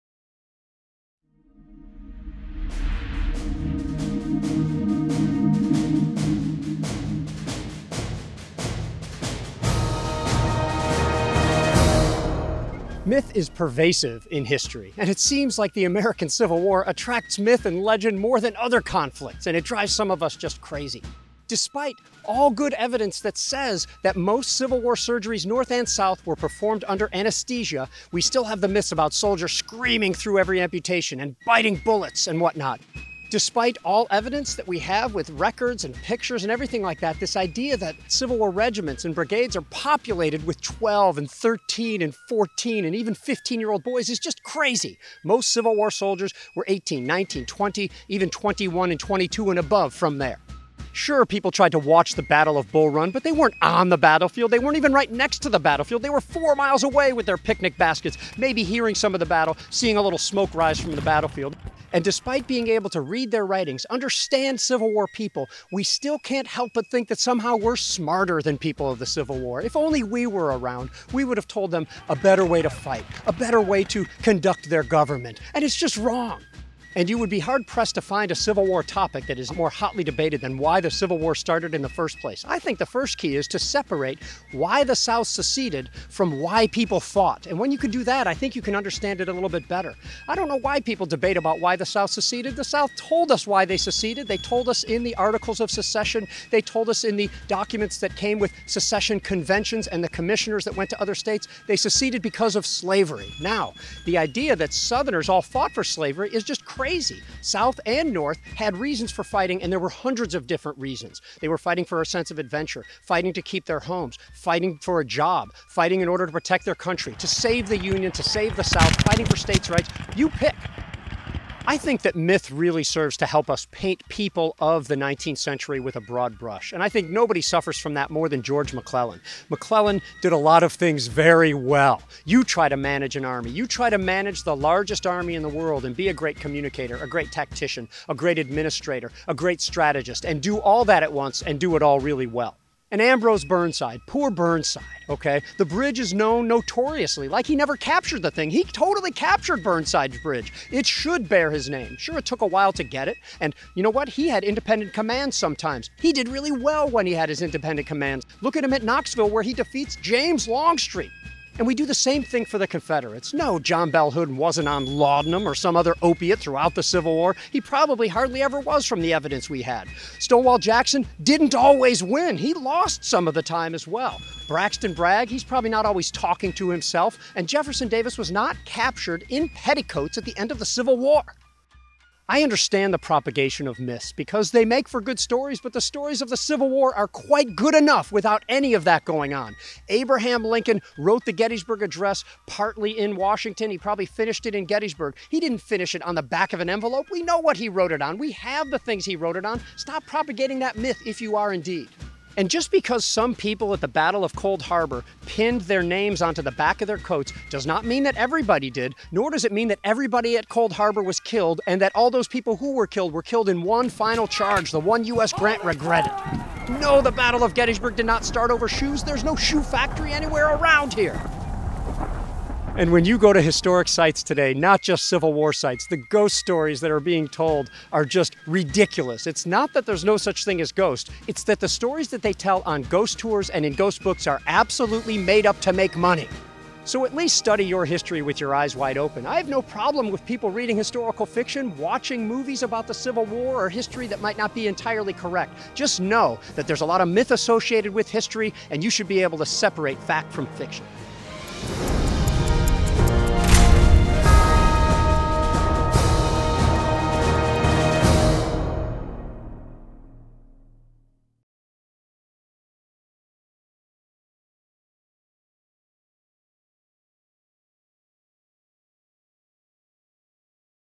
Civil War Myths: The Civil War in Four Minutes
Garry Adelman of the American Battlefield Trust details the most common misconceptions about the Civil War including why did soldiers fight, was George McClellan really that bad of a general and what is up with all of those Civil War ghost tours?